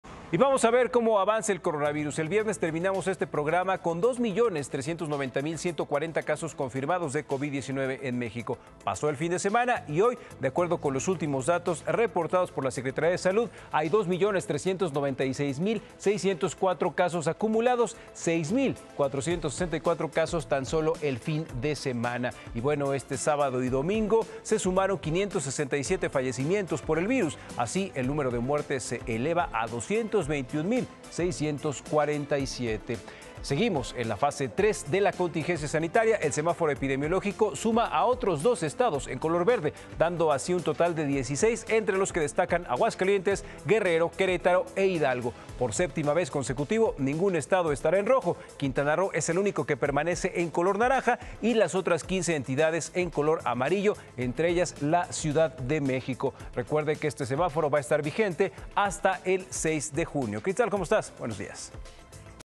Reporte covid-19: Suman 2, 396, 604 casos acumulados en México
La Secretaría de Salud informa esta noche que el número de fallecimientos provocados por covid-19 llega a 221 mil 647, ya que se tiene el registro de 50 muertes más; mientras que los casos estimados alcanzan los 2 millones 582 mil 573.